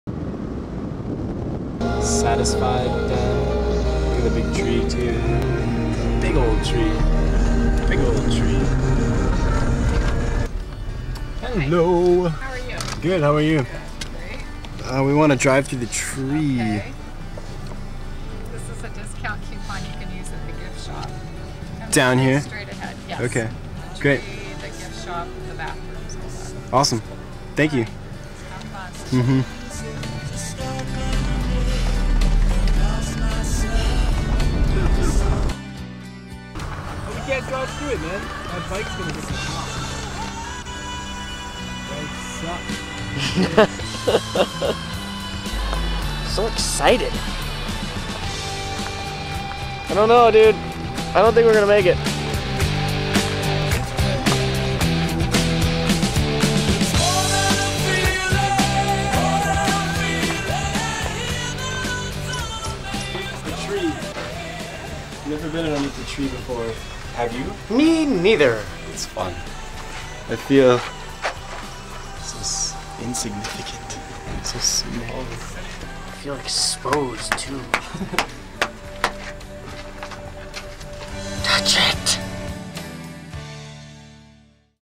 Drive Thru Tree
California use to have an awesome tree, tourism took over and crazy people chain-sawed through it so parents and their screaming kids could drive though it. Unfortunately the tree got sick after getting sawed through and no longer exists... but this sweet video does!